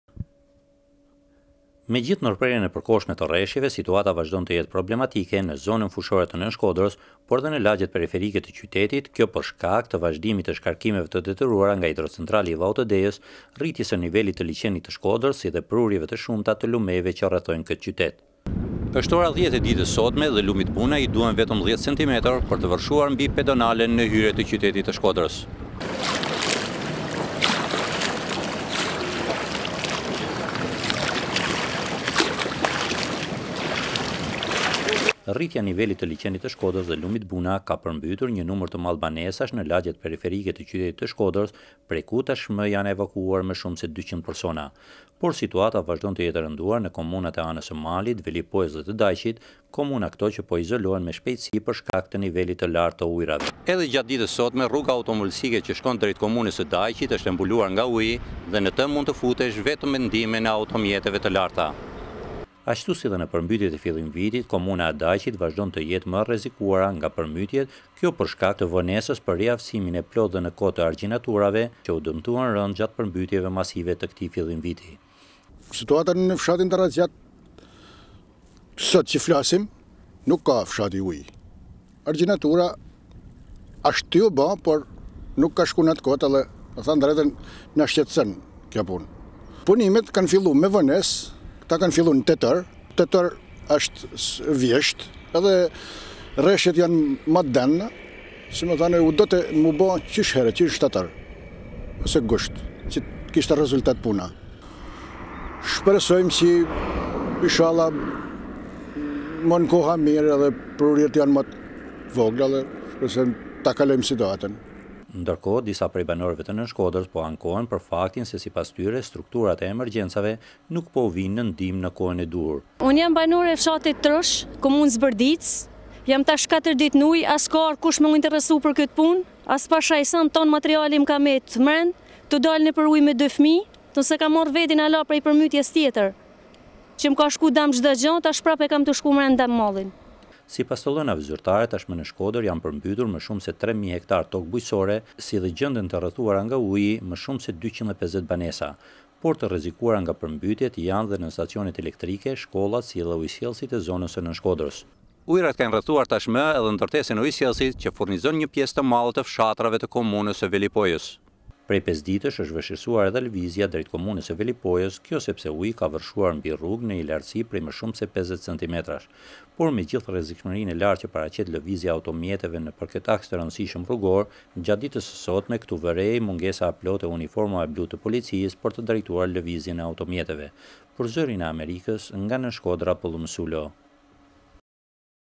Shkoder, Situata e permbytjeve
Megjithe ndërprerjen e përkohshme të rreshjeve, gjatë ditës së sotme, situata e përmbytjeve në Shkodër nuk ka njohur përmirësim. Niveli i ujit në Liqenin e Shkodrës vazhdon të  rritet, duke rrezikuar lagjet periferike të qytetit si  dhe zonën fushore të Nënshkodrës, ku tashmë gjenden nën ujë më shumeë se 3 mijë hektarë toke bujqësore.